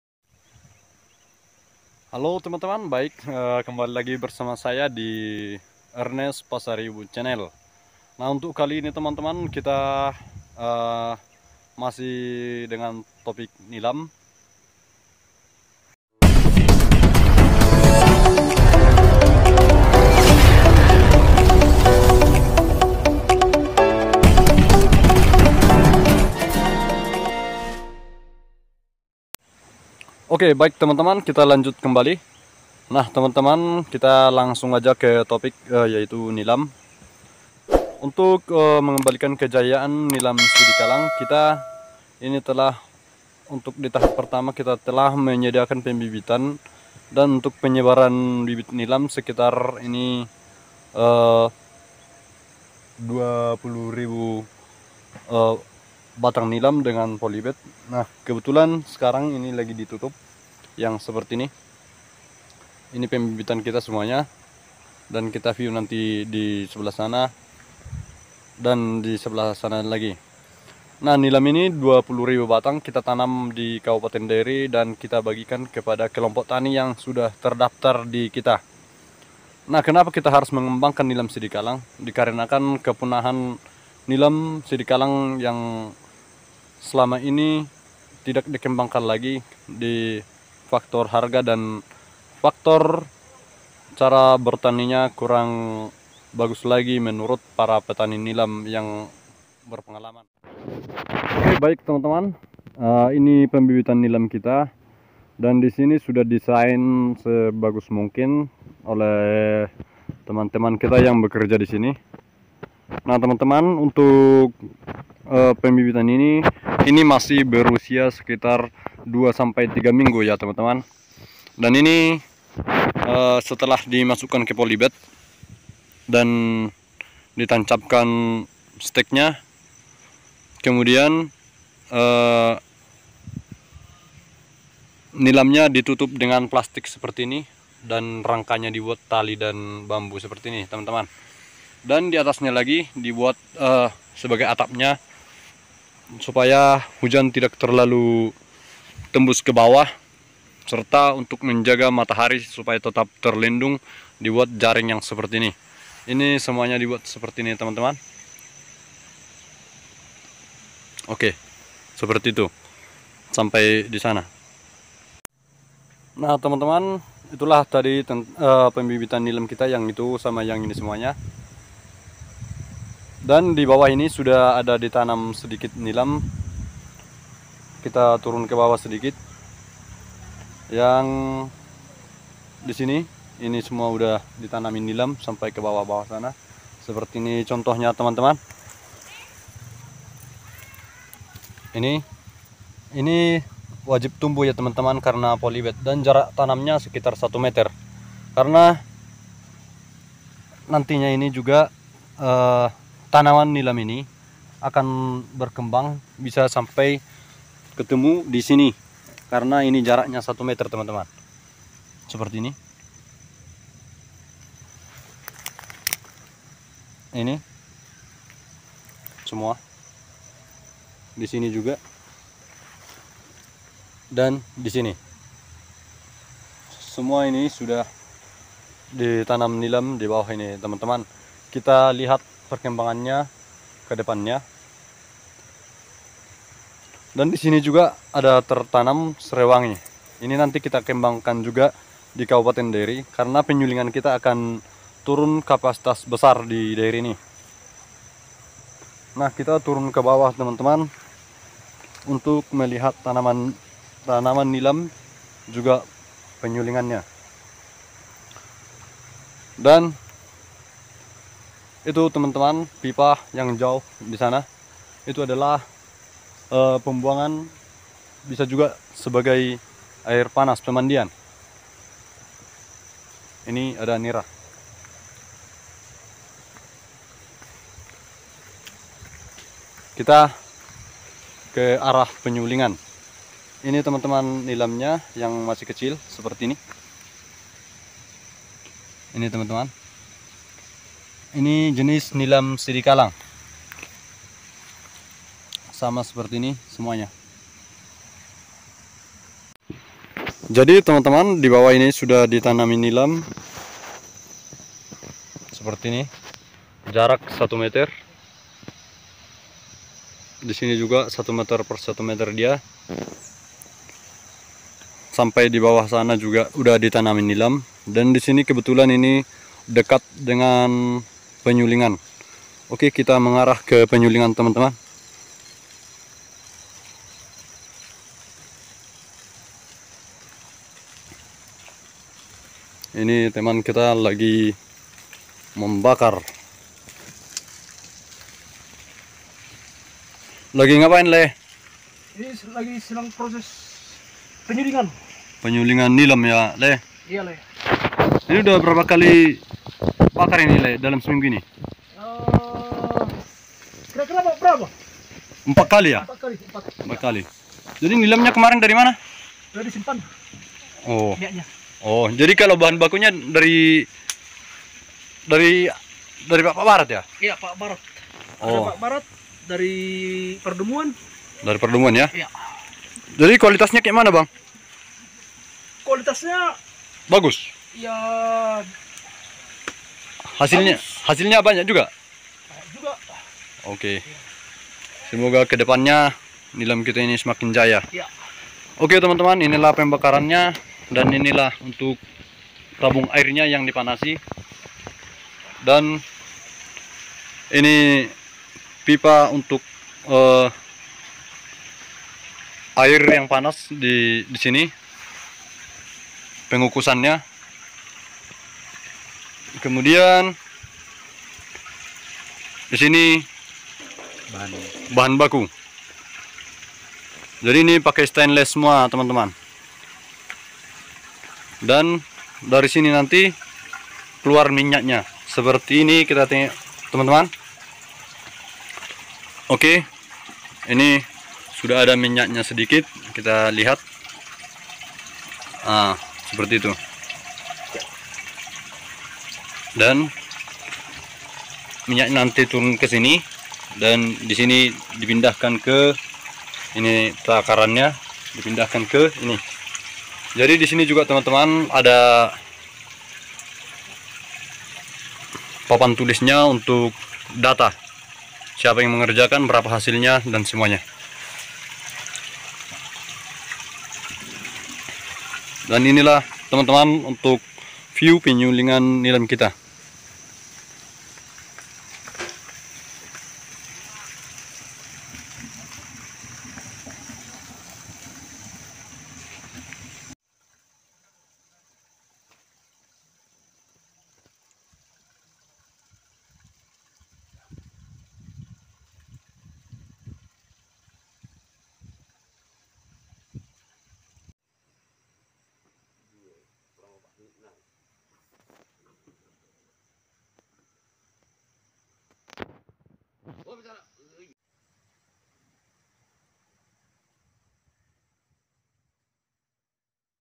Nilam Sidikalang akankah Kembali Jaya di Sidikalang?..
apakah Nilam sidikalang akan jaya di Sidikalang
yang Ingin bergabung bertani Nilam khusus Dairi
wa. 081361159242